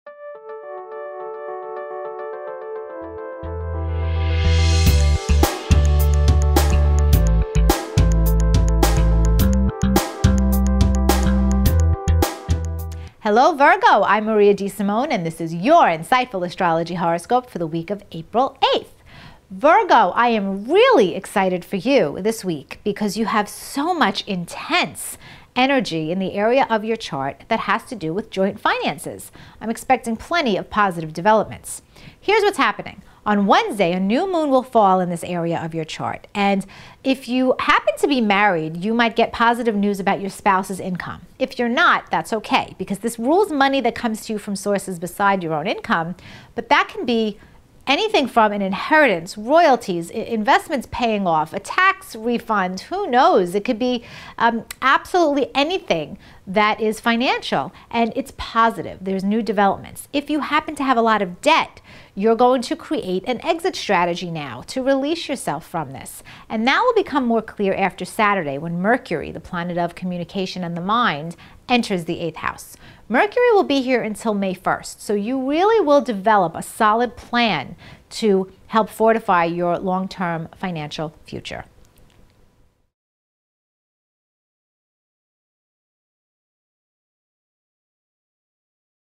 Virgo Week of April 8th 2013 Horoscope
Virgo: August 24th -- September 22nd
Insightful Astrologer Maria DeSimone presents your weekly horoscope for the Week of April 8th  for the sign: Virgo